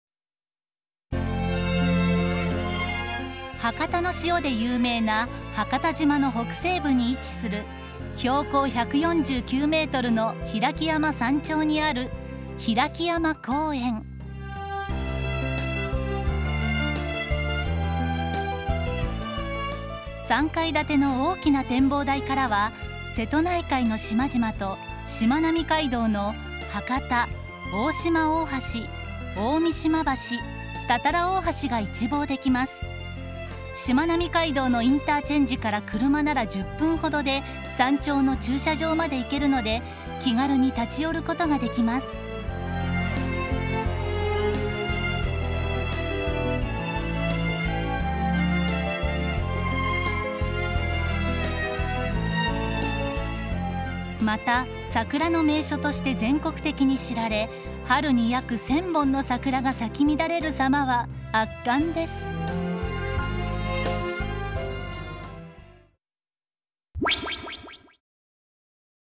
Hirakiyama Park - Setouchi Shimanami Kaido Tourist Guide See a video
Please show the details in "Machi-log Setouchi Shimanami Kaido Tourist Guide"

Hirakiyama Park is located on the peak of 149m tall Mt. Hirakiyama, in the northwest of Hakata, an island known for its salt.
From the three-story observation facility, look out over the islands of the inland sea, and the many bridges of the Shimanami Kaido.
The mountaintop parking lot is just a ten-minute drive from the Shimanami Kaido interchange, so it's easy to stop by.
"The mountain is known nationwide for its beautiful cherry blossoms.
The sight of approximately 1000 trees in bloom is truly impressive."